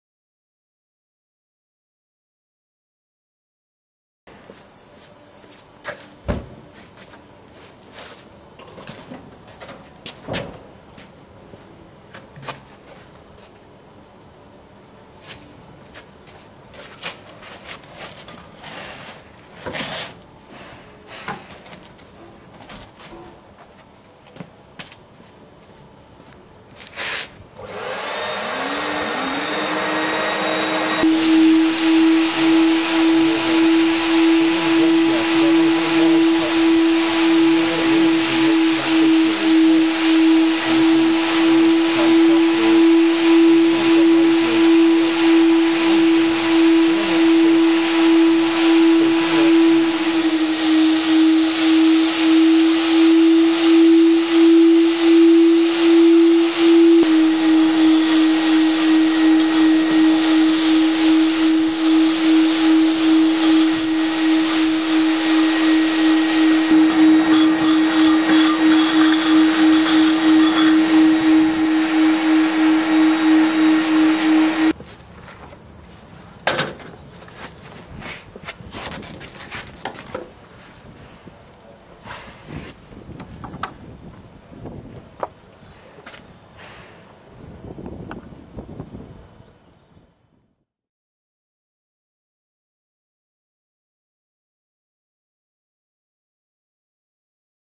Fill your own drums with the Big Brute Interceptor
The Big Brute Interceptor lets you suck up waste into your own steel drums, whatever their size. Ideal for recovering or decanting waste into your own vessels for easy disposal or storage. Use with your Big Brute Industrial Vacuuum Cleaner.